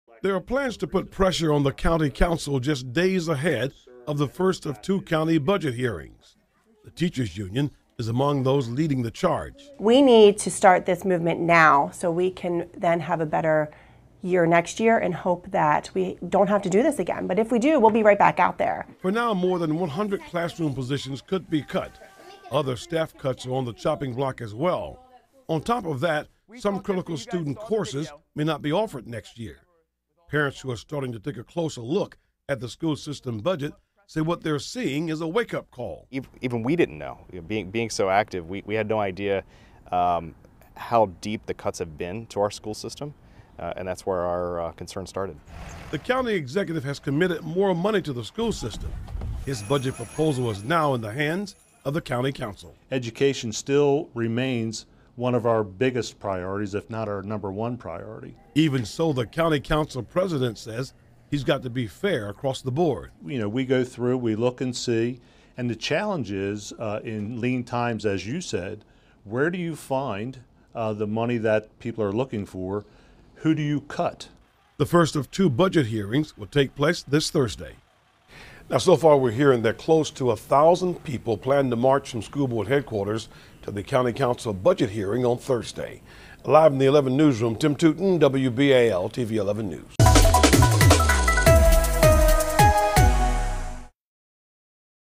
Budget proposal could leave Harford County schools $5M short
Harford County Public Schools could come up $5 million short in the county's proposed budget. There are plans by parents and teachers to put pressure on the County Council just days ahead of the first of two county budget hearings. The teacher's union is among those leading the charge.